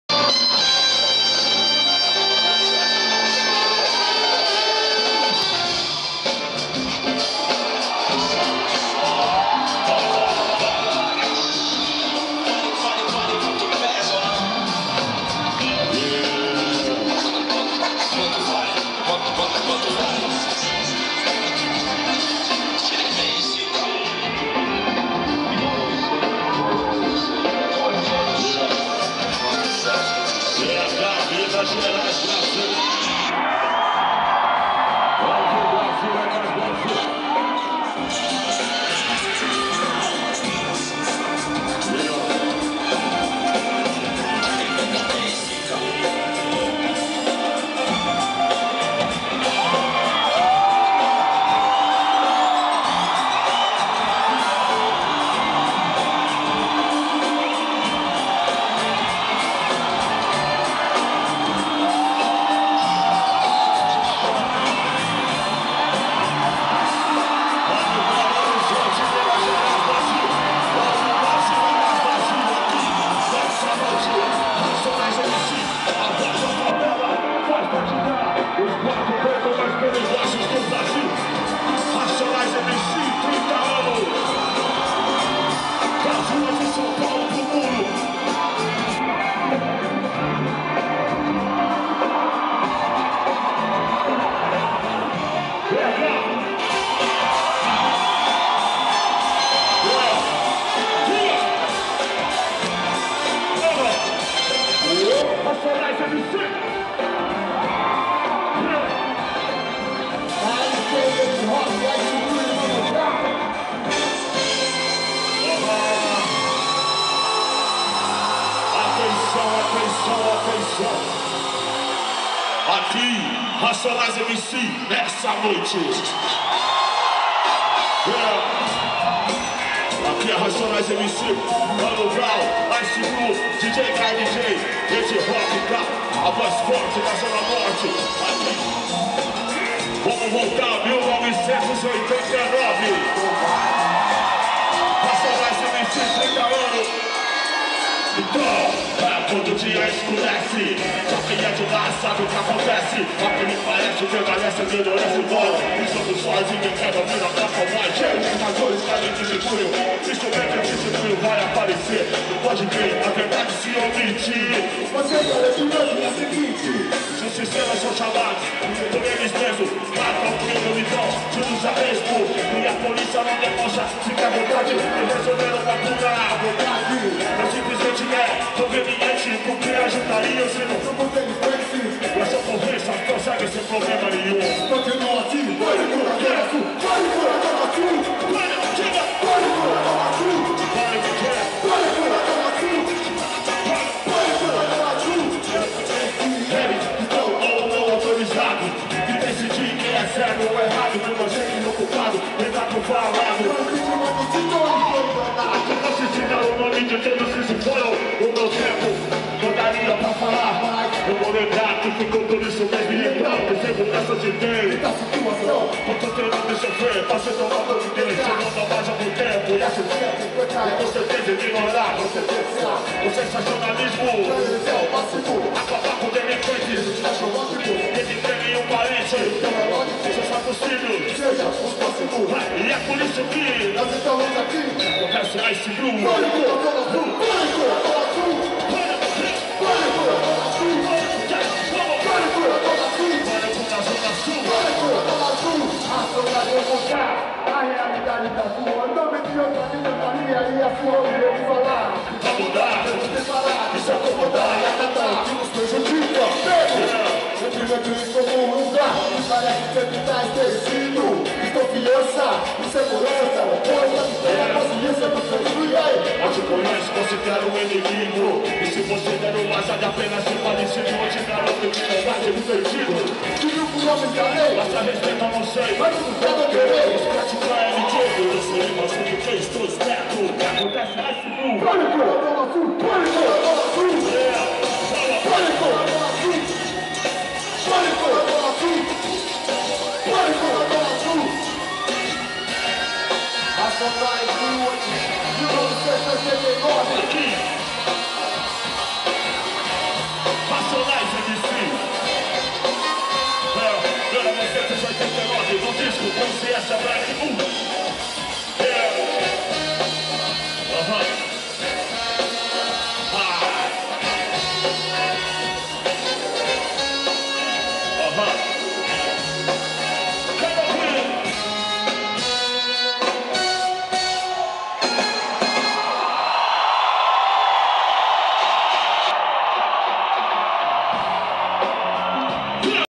Racionais em Belo Horizonte Tour 2019 - 06 de 27
Racionais MC's
KM de Vantagens Hall - Belo Horizonte/MG

Um dos maiors grupos de rap do país volta aos palcos para celebrar os 30 anos de carreira. O Racionais MC’s apresenta a turnê Racionais 3 Décadas – Tour 2019 no Km de Vantagens Hall, dia 14 de setembro, a partir das 22h30.
A banda é composta por Mano Brown, Edi Rock, Ice Blue e DJ KL Jay.